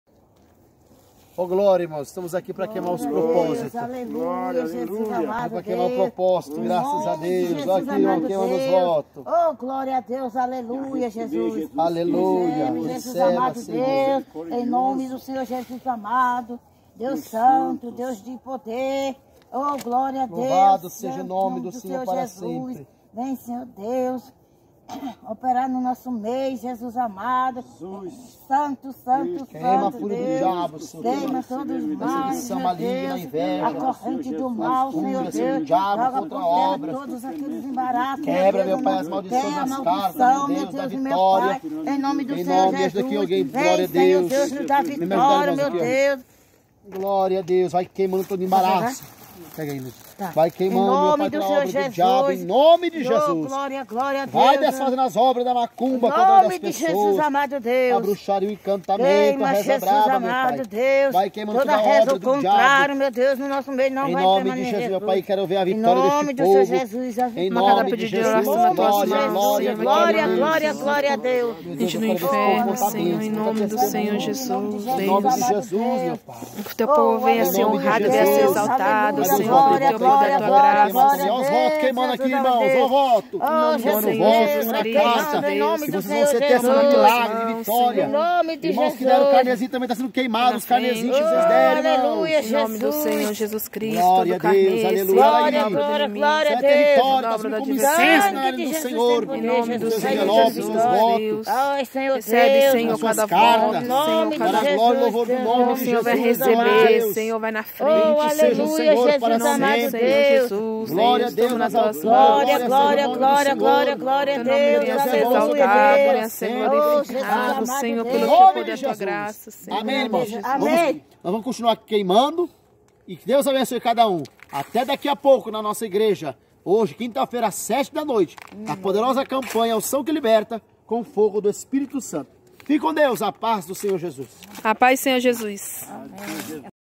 Pastor Andrey Damasceno Queimando os Pedidos de Oração no MONTE
Pastor Andrey Damasceno Queimando os Pedidos e votos no monte.